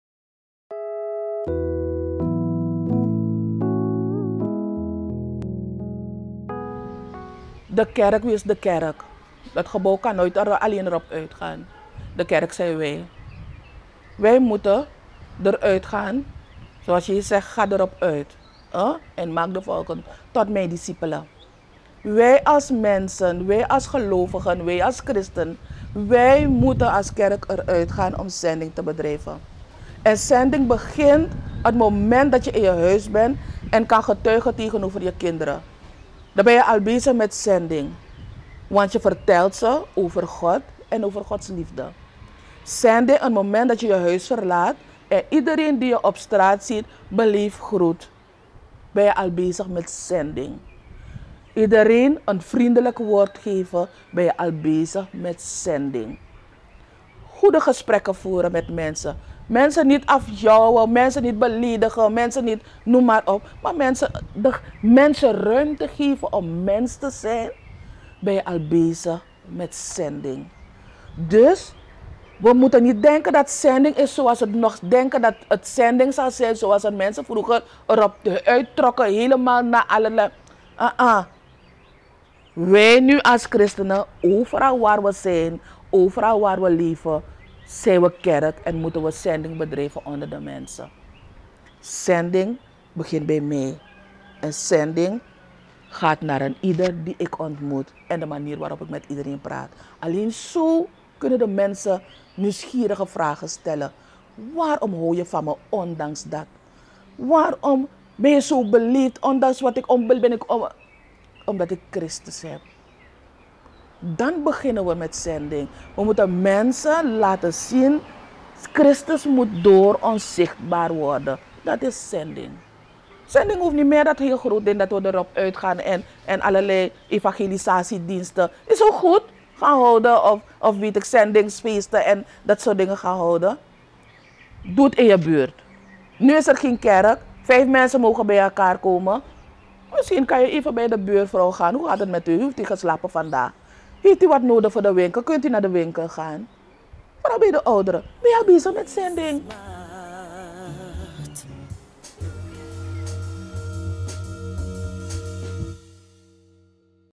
Zr. Rozie: "De kerk zijn wij.. wij moeten erop uitgaan"
De EBG(S) is ontstaan door middel van het zendingswerk. Zendelingen die vanaf 1722 de wereld rond zijn gegeaan en de blijde boodschap hebben verkondigd. Hoe belangrijk is het zendingswerk in deze tijd en wordt er genoeg aandacht aan besteed?